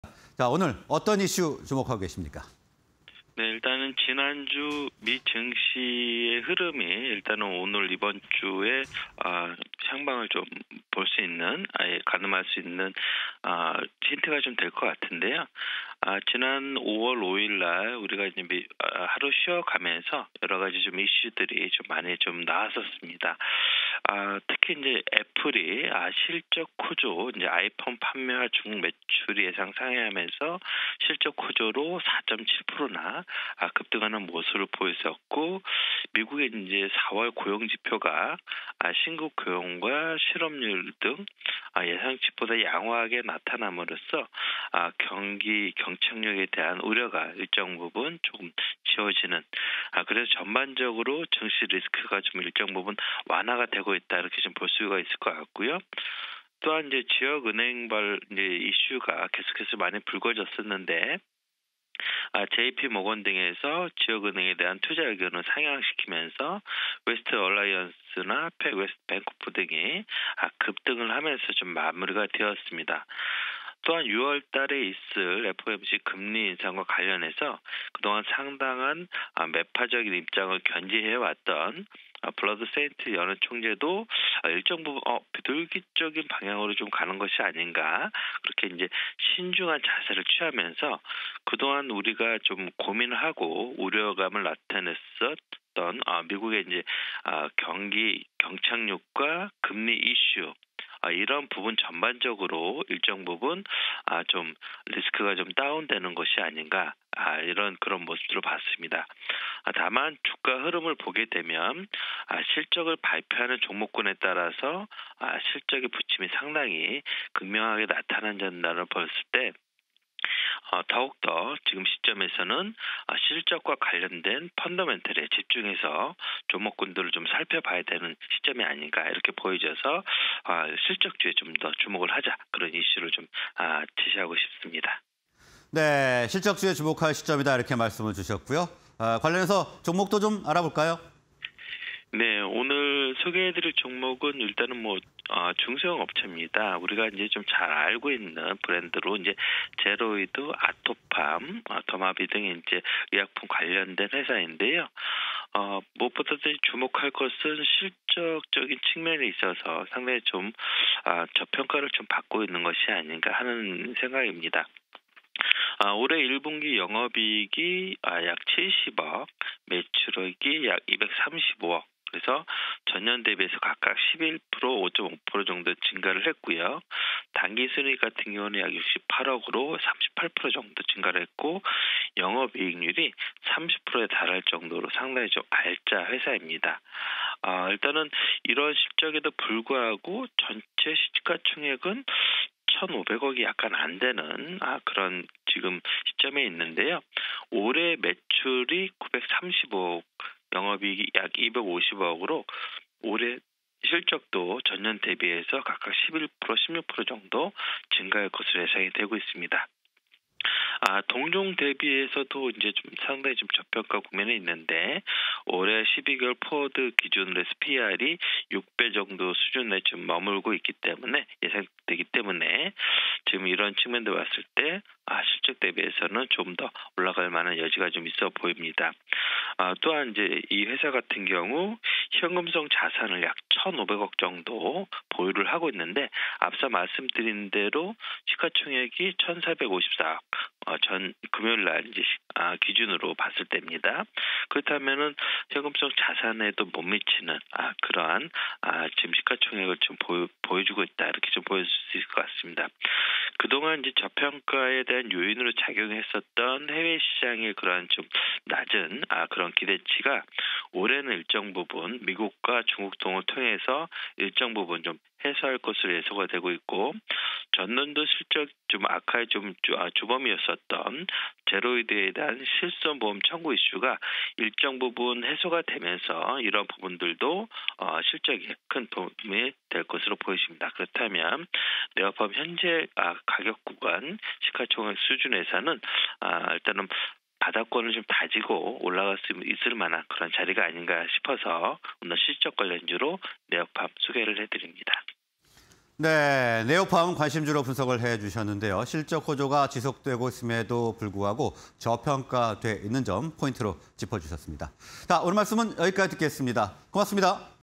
네오팜(092730)... 증시 리스크 완화… '실적주' 주목할 시점?_투자 인사이트 (20230508)
Q. 증시 리스크 완화… '실적주' 주목할 시점?

네오팜(092730)
- 1분기 영업익 70억… 전년비 11.1% ↑
- 올해 실적 호조 지속 전망
- PER 6배 수준으로 저평가 국면
- 제로이드(매출 30% 비중) 이슈 해소
- 현금성 자산 1500억원 상당 보유
- 올해 미국·중국 등 해외 성과 기대